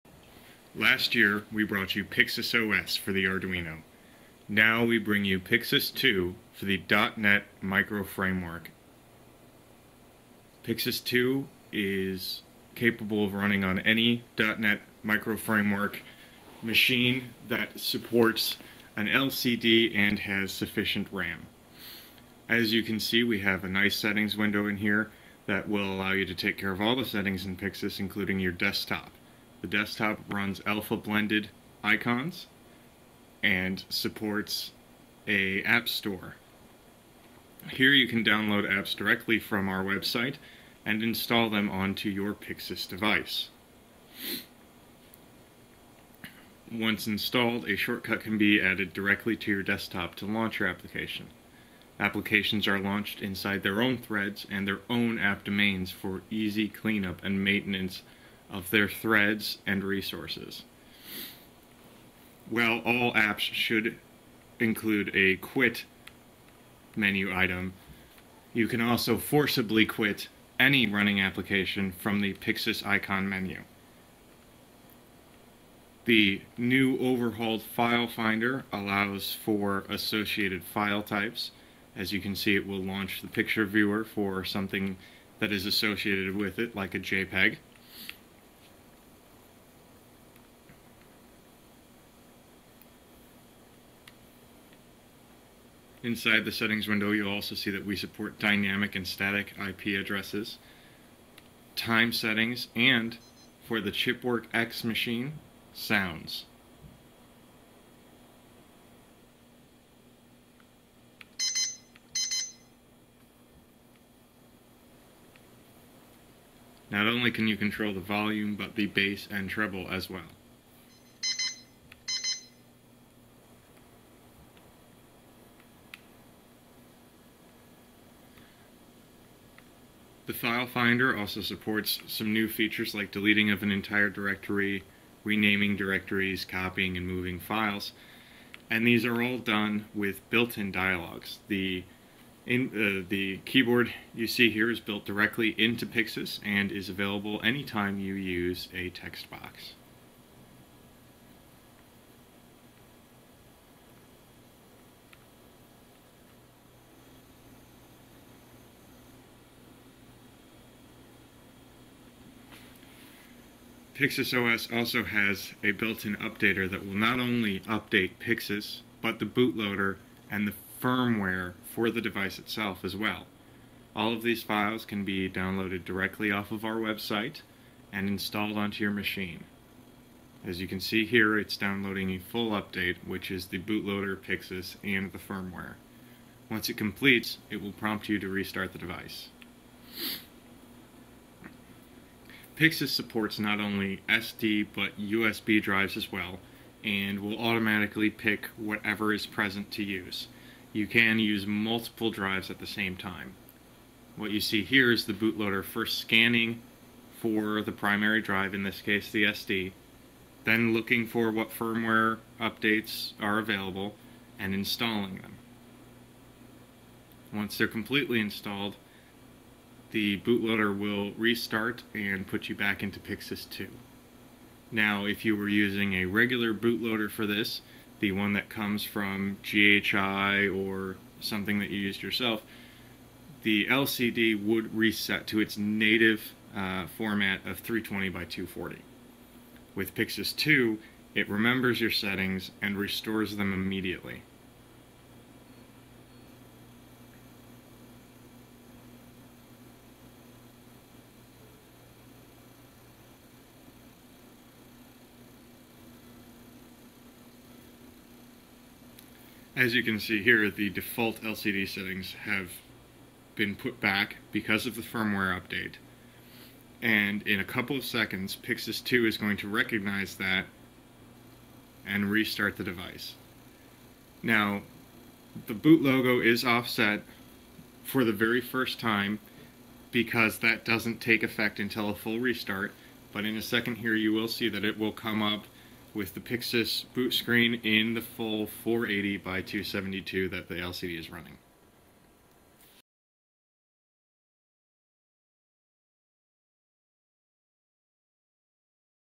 Pyxis 2 (Production Release)
The latest version of Pyxis 2 (which was reviewed by Microsoft is now available in full production release!

Launch applications, access the App Store, use the enhanced File Finder, enjoy alpha-blended icons and much more!